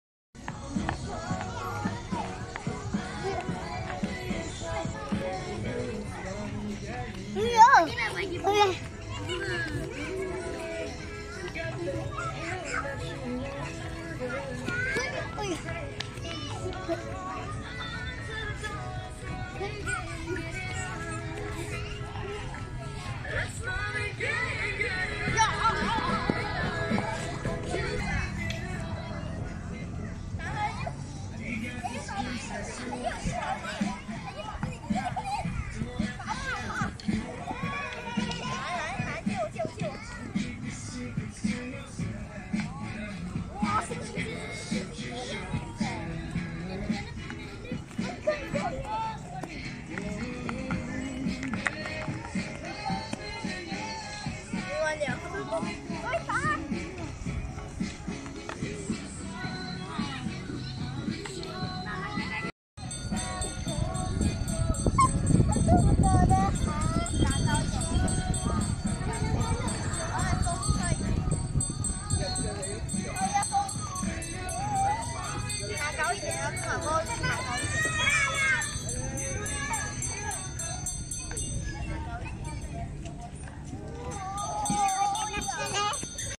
Children playing at the Park | playtime with bubble | polo ground | 快乐周末 | 公园玩乐 | 吹泡泡
polo ground - a good place to do your daily walking and jogging in the heart of the city, Ipoh. Polo Ground 休闲公园，位于城市的中心，适合一家老小运动，玩乐的好地方。